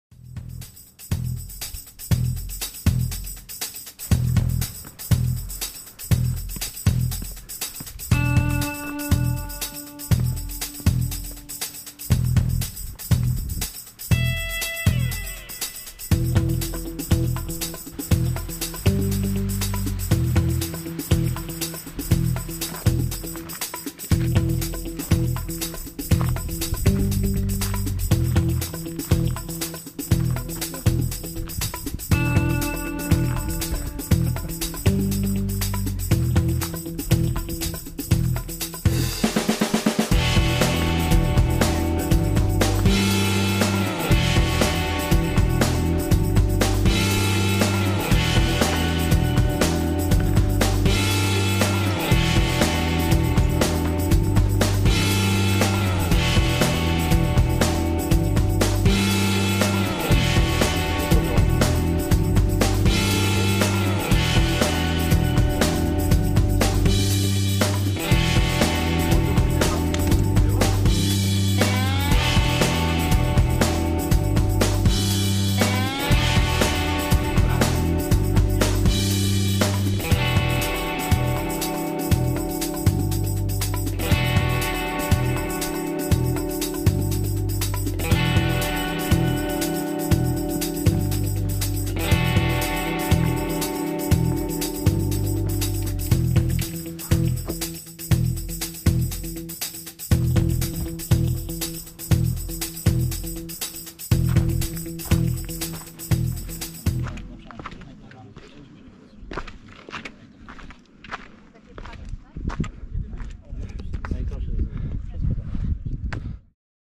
Orla Perć 2022
Krótka relacja z przejścia sławnego szlaku.
22/07/2022